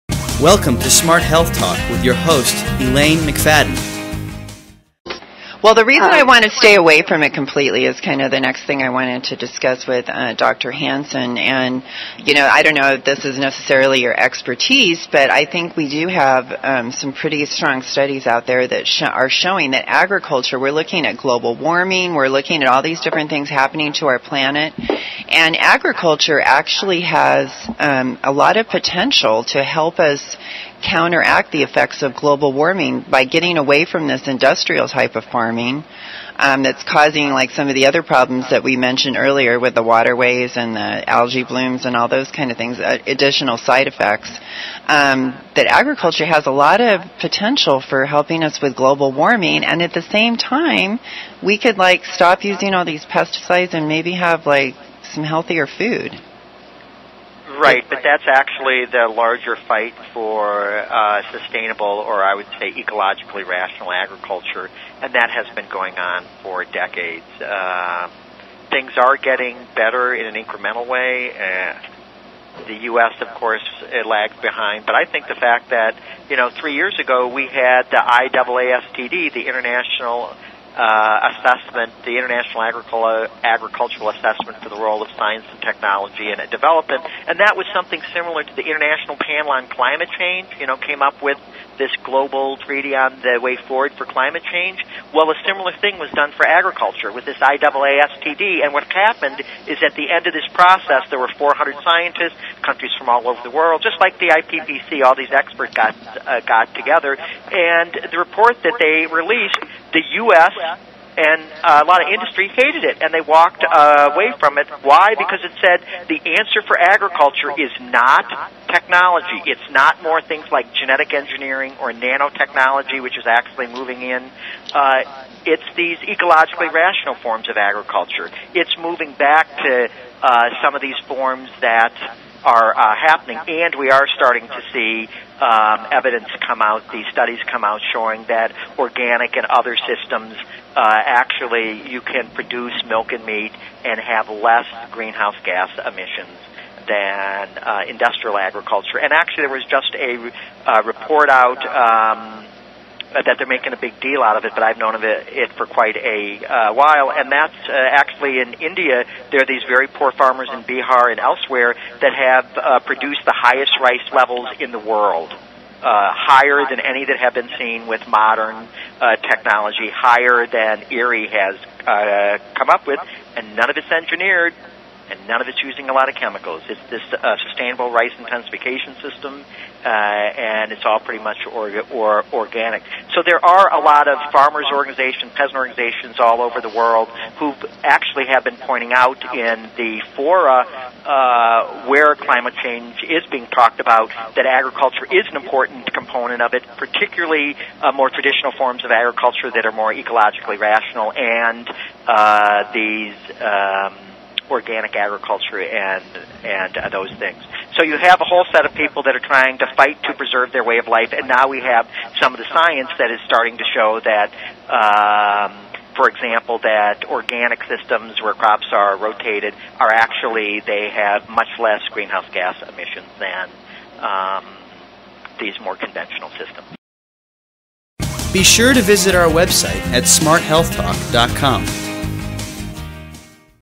Dr. Hansen on how Organic Agriculture Reverses Climate Change
Because conventional agriculture is pumping large amounts of CO2 in the air and is therefore A MAJOR CONTRIBUTOR TO THE WARMING OF THE PLANT GLOBALWARMING, it is ORGANIC FARMING that has been keeping our atmosphere in balance even during times like the industrial revolution when pollution was at it's worst but we were not destroying key living organisms in soil that keep CO2 from escaping into the air and instead keeps it locked in the soil.

Pesticides/herbicides have destroyed 90% key mycorrhiza fungi!  There is a long list of other horrible things pesticides are doing to us as humans and our planet.